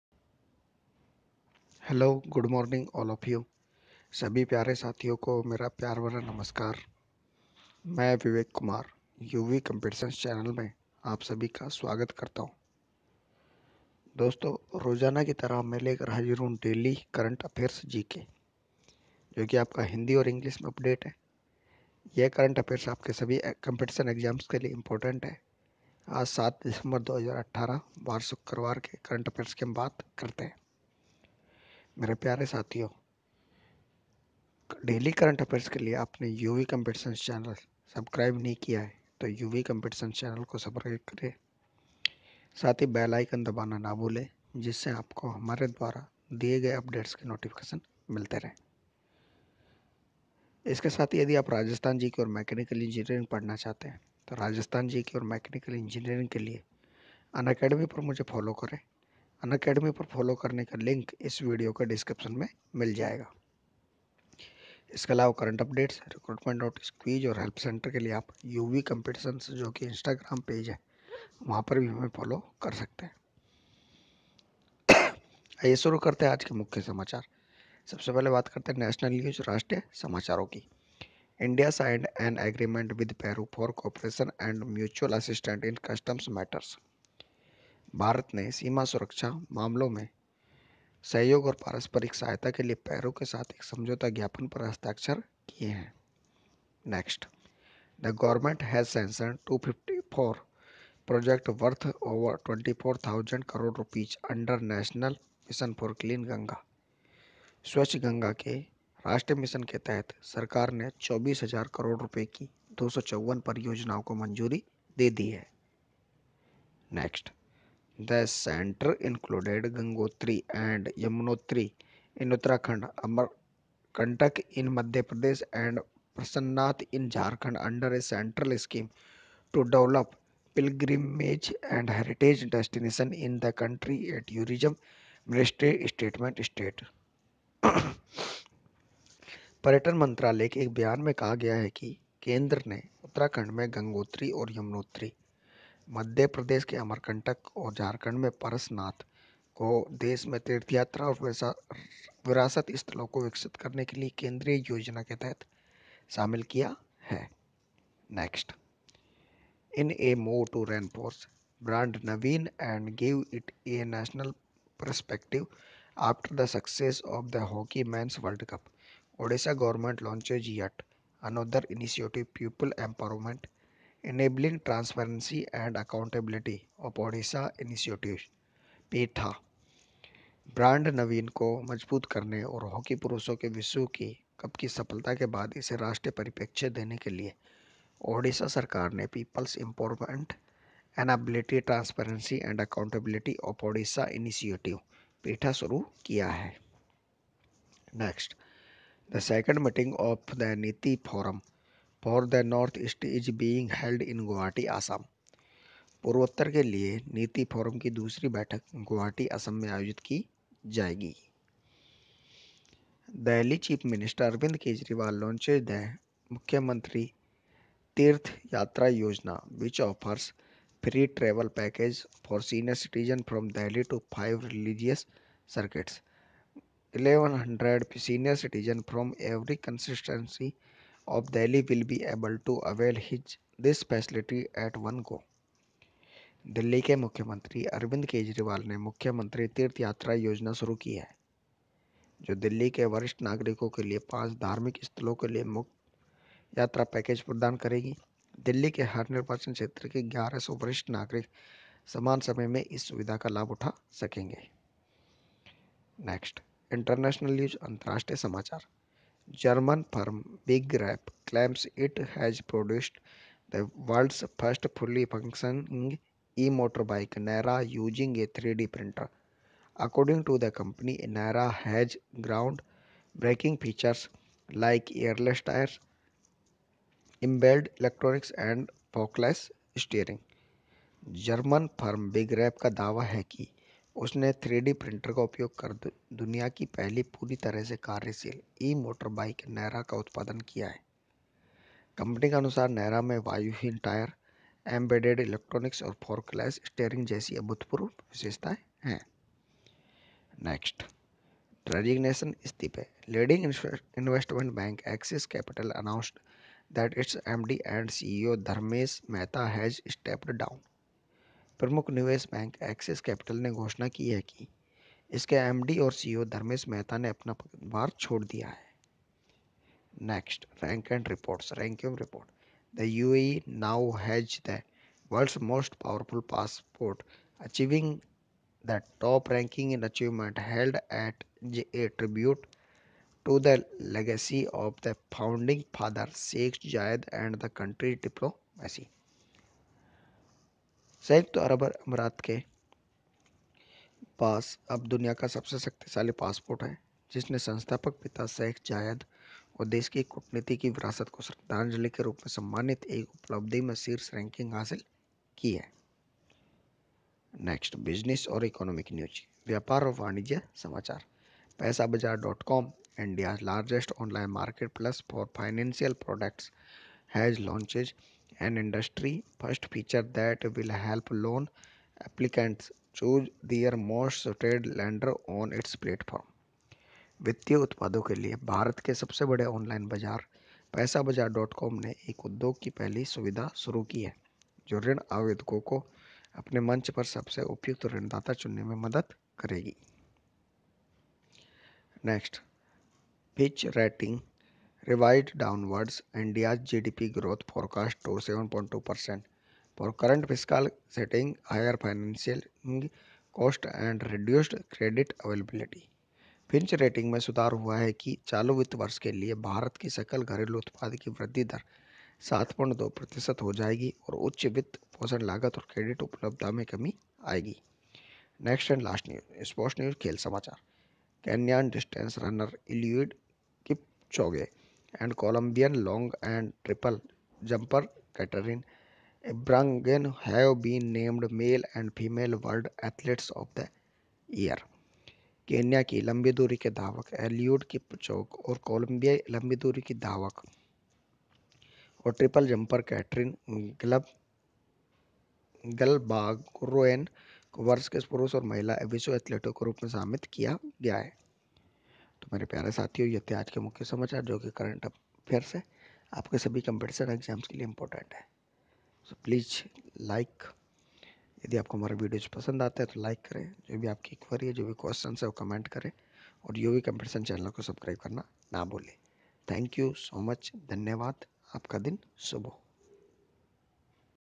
Daily Current Affairs GK updates 07 December 2018 in Hindi & English for UPSC, SSC, Banks Exams
Daily Morning Current Affairs GK updates 07 December 2018.

Downlaod PDF -